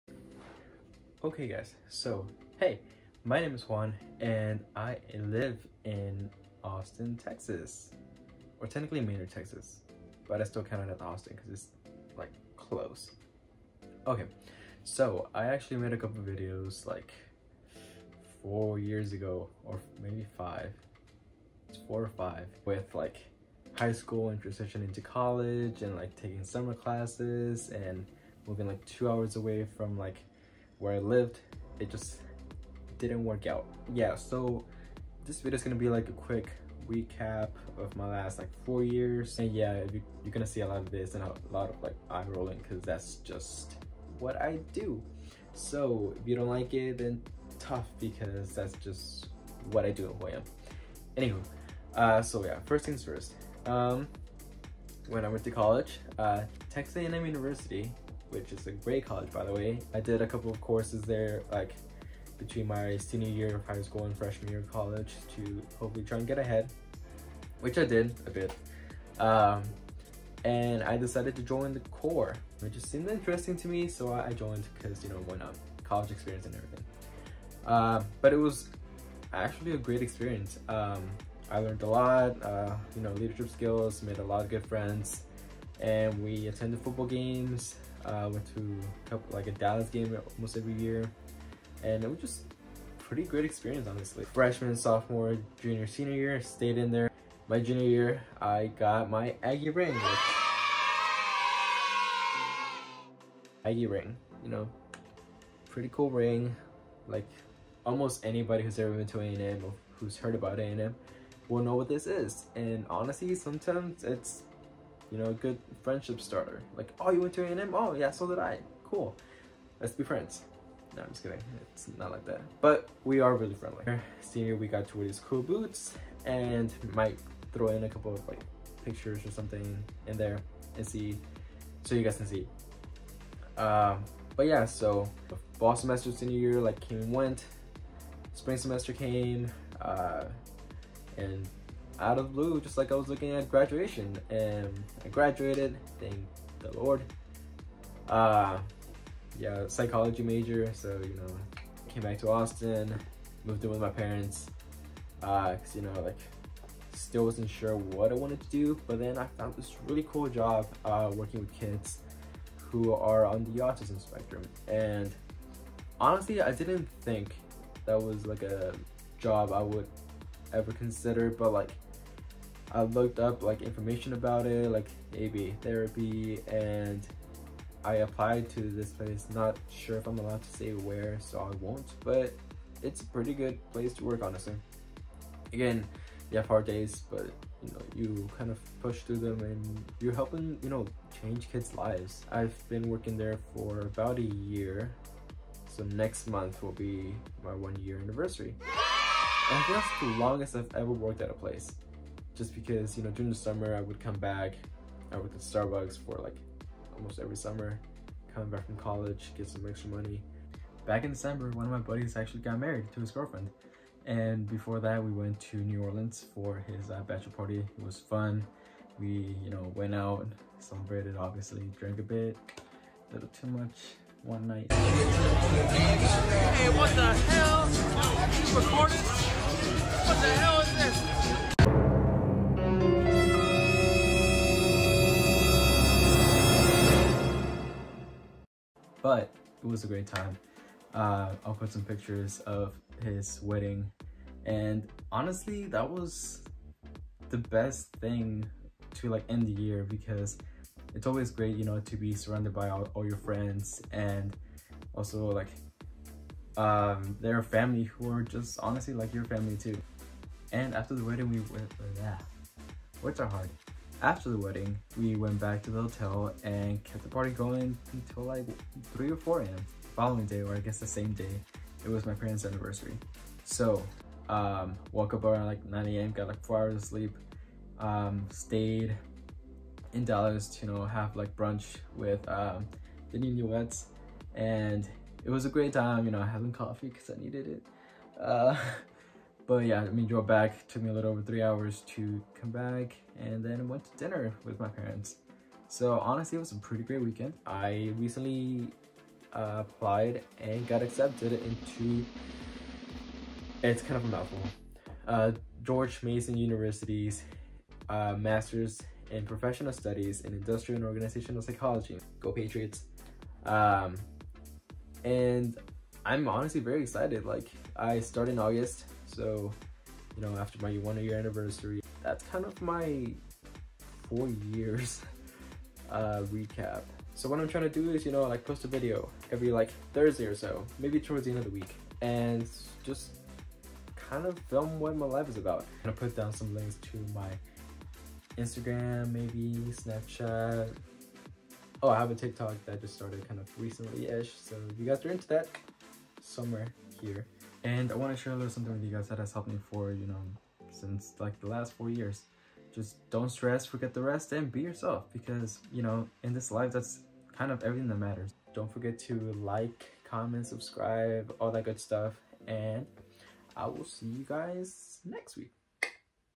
WHAT I'VE BEEN UP TO | Juan Aguilar
Little Recap of what I've doing for the last 4-5 years and hoping to enjoy this new hobby of mine.